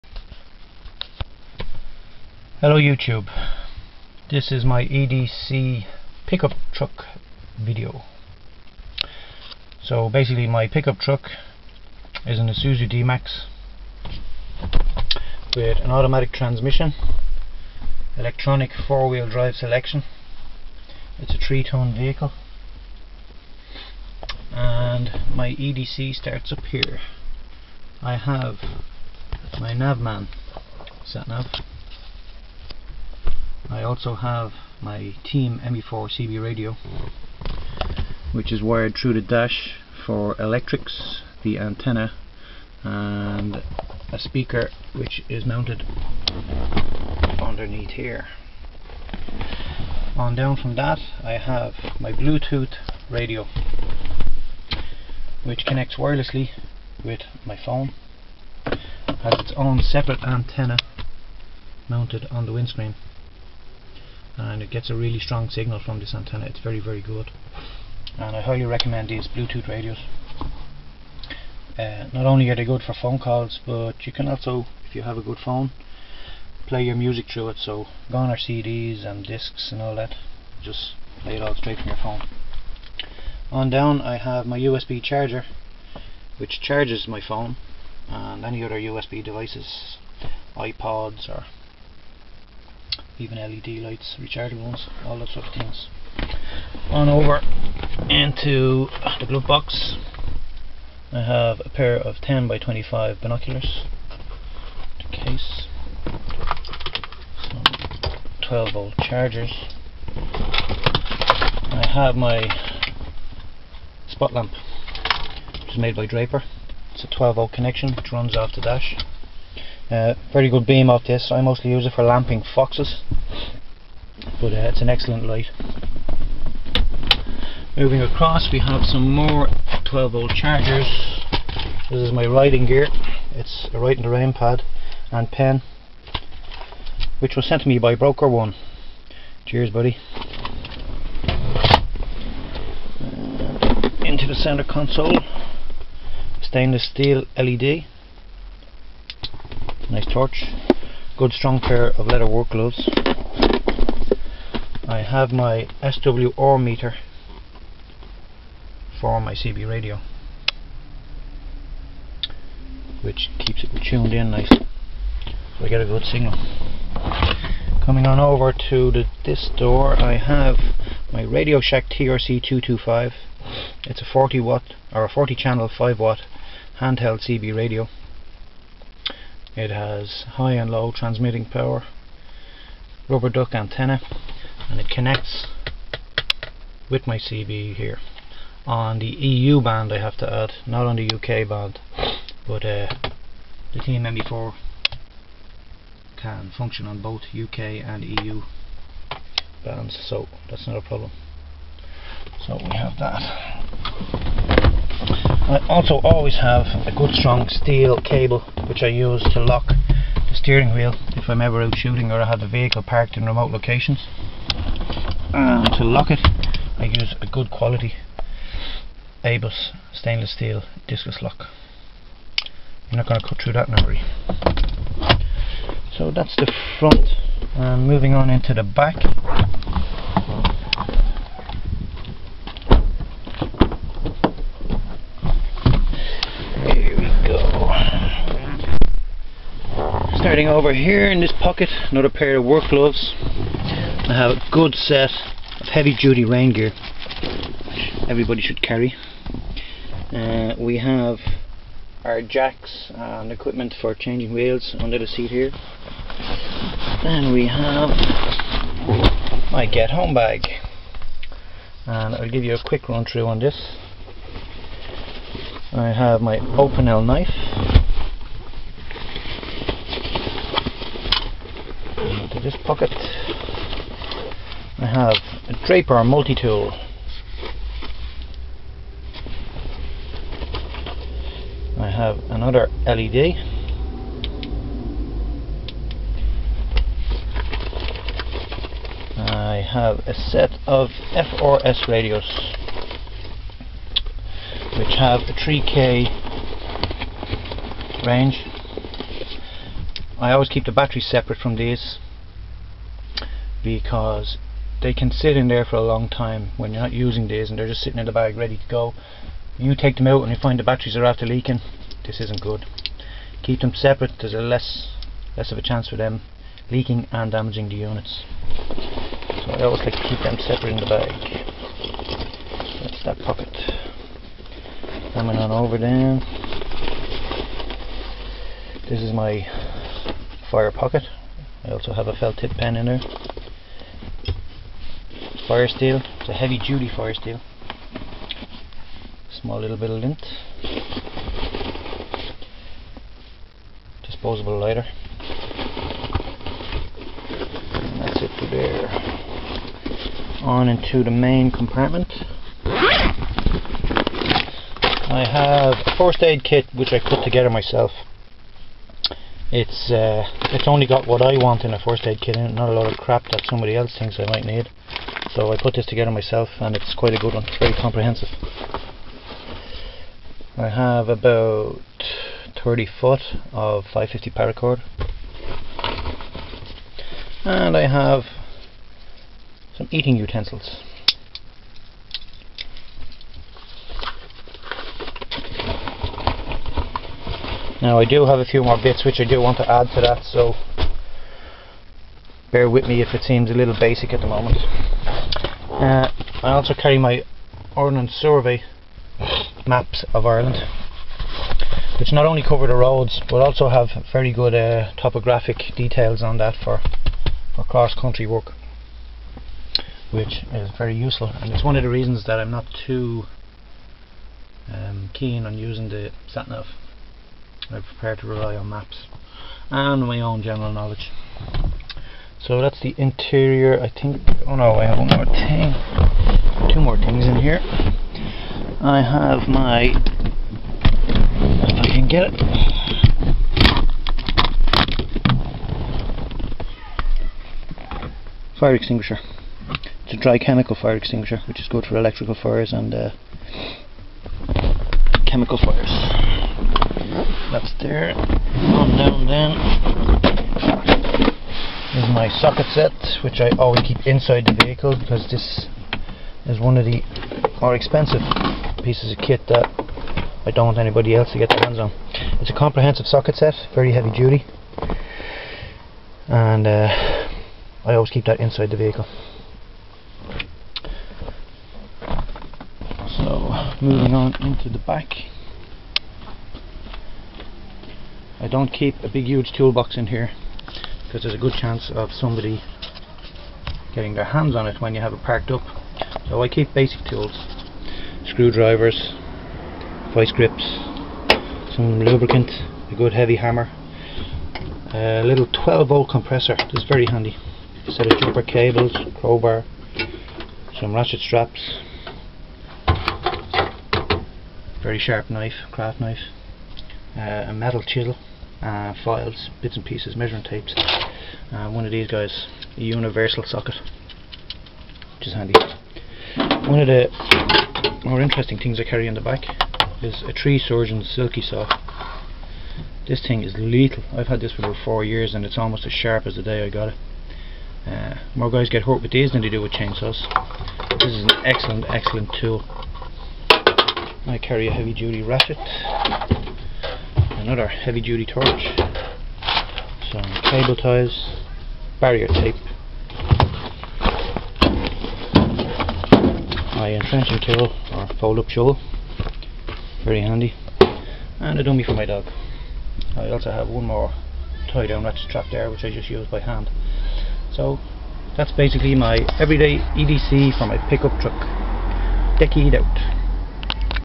Vehicle EDC.
EDC for my 2005 isuzu dmax/Rodeo pick up truck